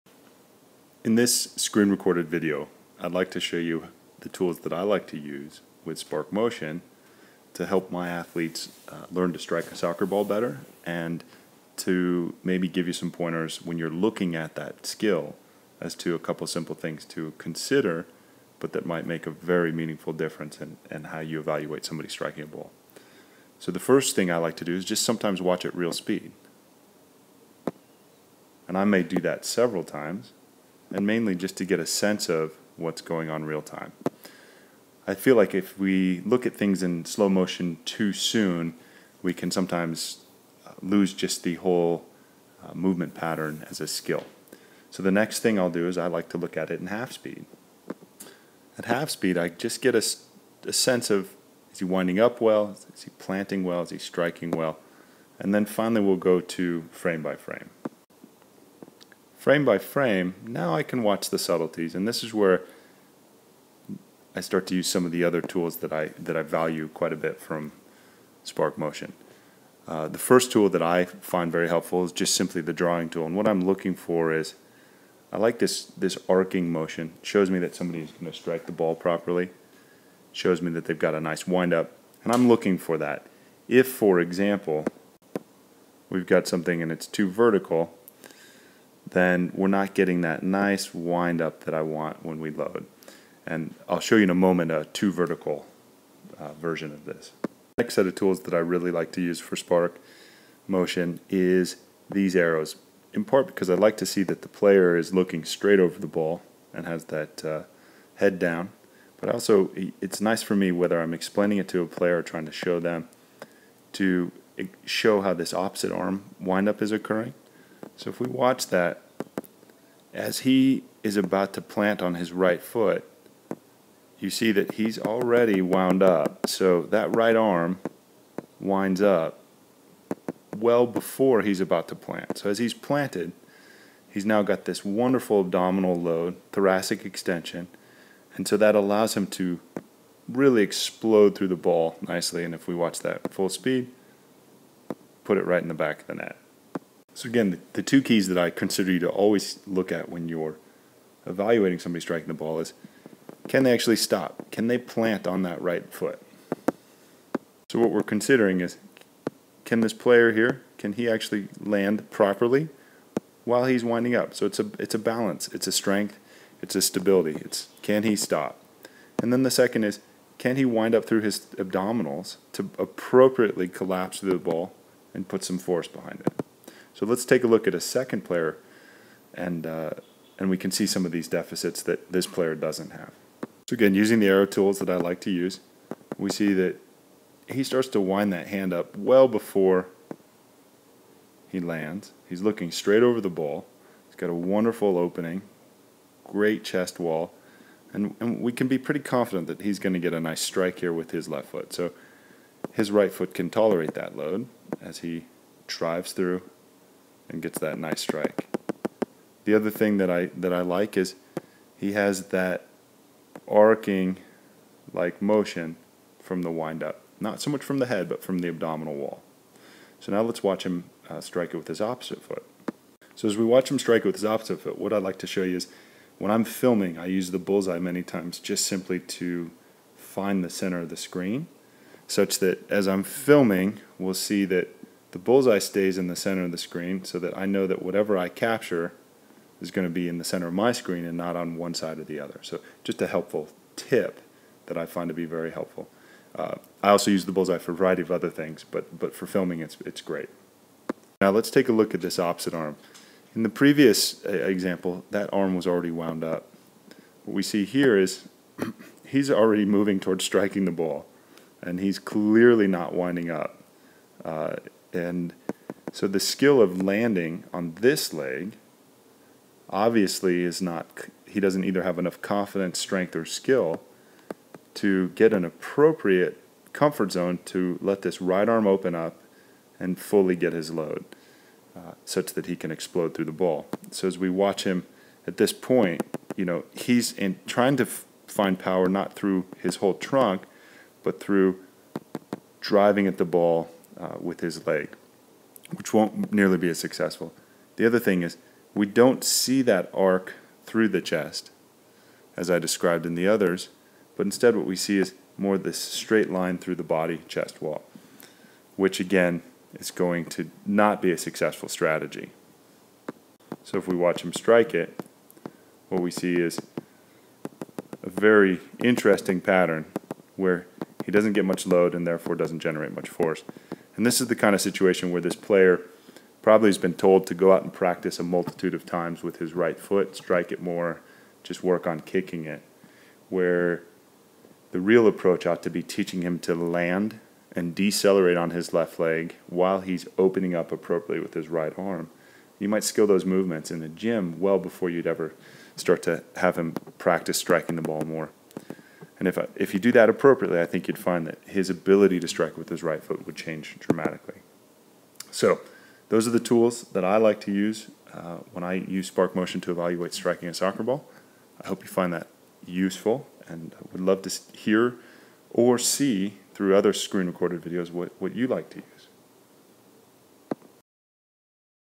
Striking or Kicking a Ball Using Spark Pro
" This video shows how CoFounder Dr. Nick Studholme uses Spark Pro's SRV tool to teach and correct kicking or striking technique in Soccer.